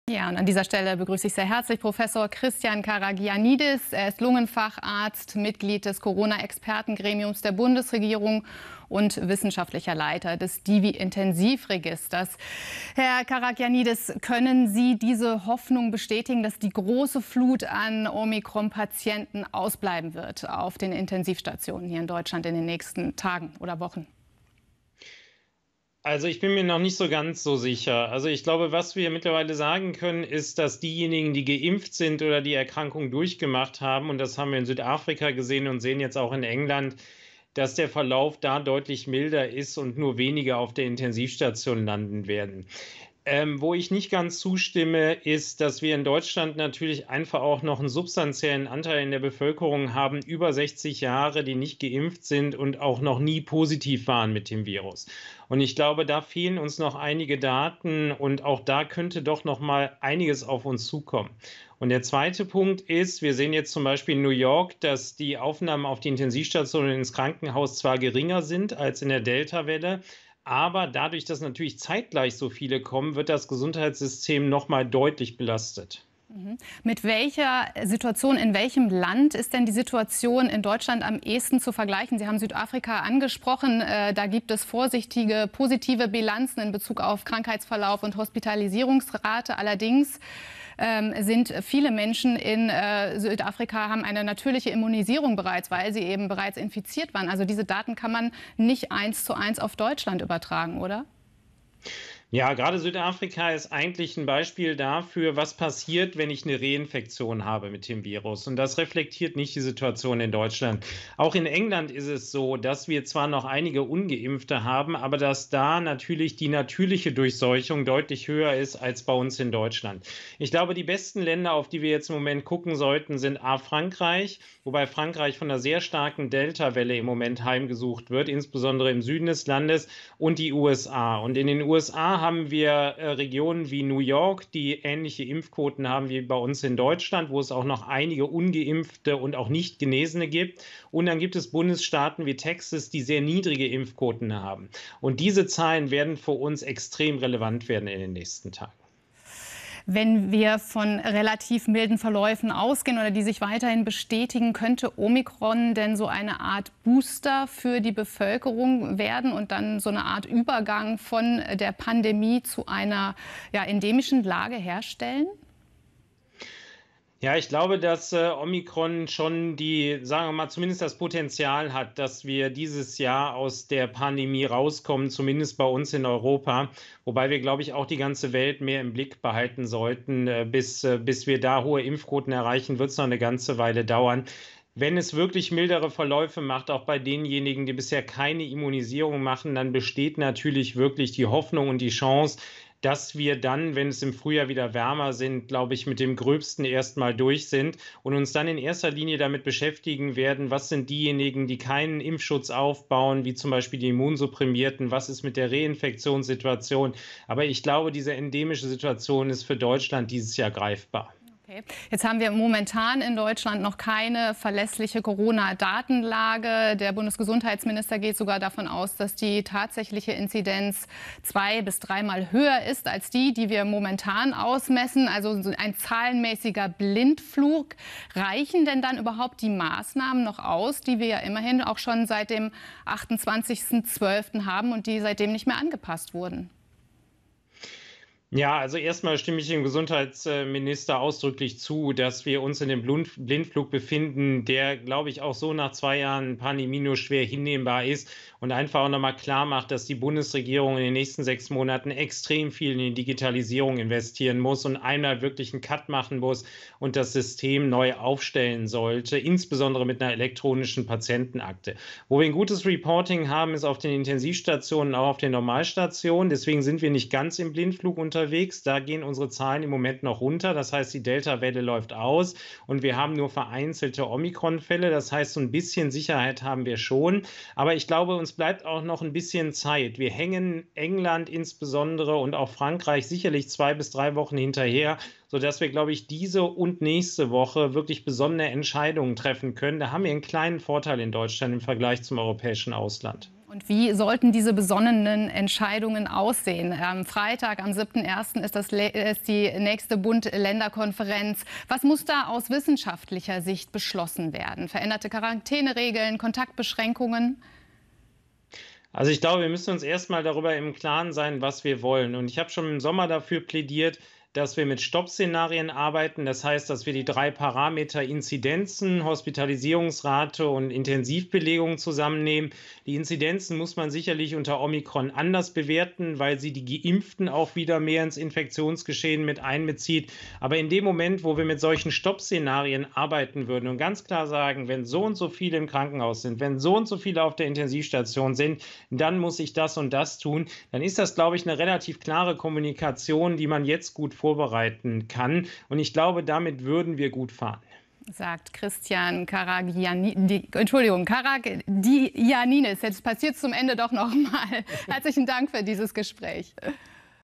Omikron: Schaltgespräch mit Prof. Christian Karagiannidis am 03.01.22
Schaltgespräch mit Prof. Christian Karagiannidis, Lungenfacharzt und Leiter des Intensivregisters der Deutschen Interdisziplinären Vereinigung für Intensiv- und Notfallmedizin (DIVI), sowie Mitglied des Corona-Expertengremiums der Bundesregierung, zur aktuellen Corona-Lage und zur Situation auf den Intensivstationen.

Damit Sie immer auf dem neusten Stand bleiben, folgen Sie uns auf unseren Social Media Kanälen: